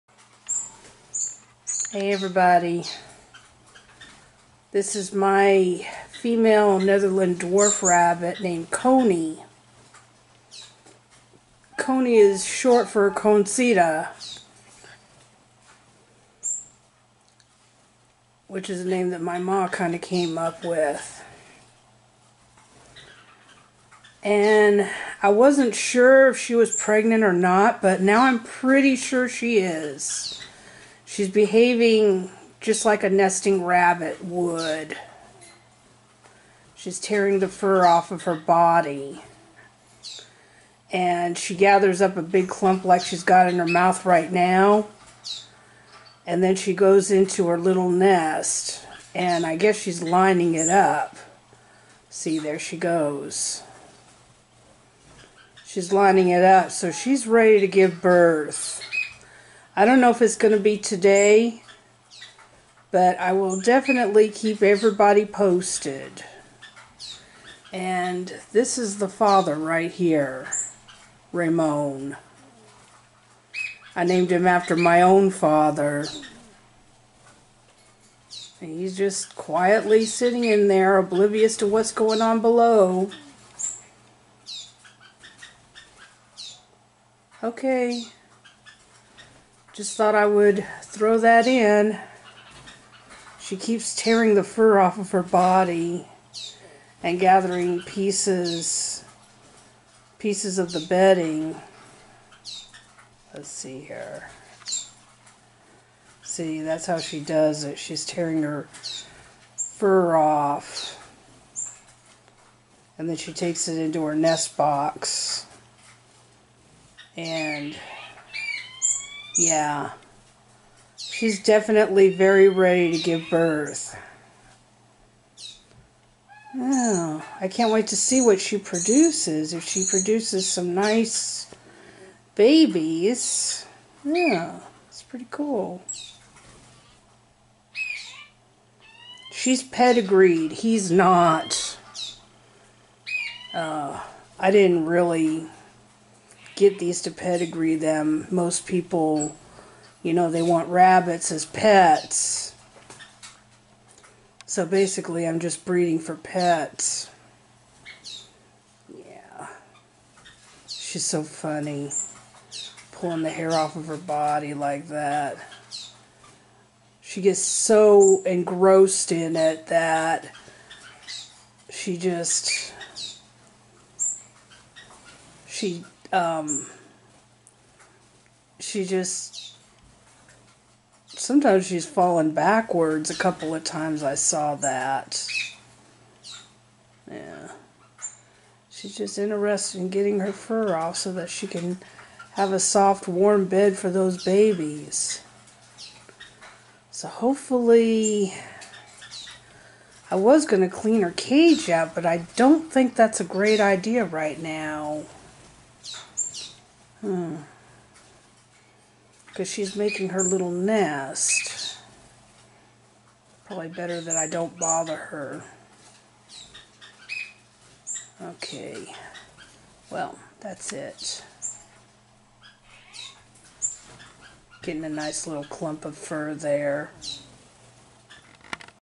Coney in Nesting Phase
My pedigreed female Netherland dwarf rabbit is about to give birth. Right now, she is pulling the hair off her body to line her nest with.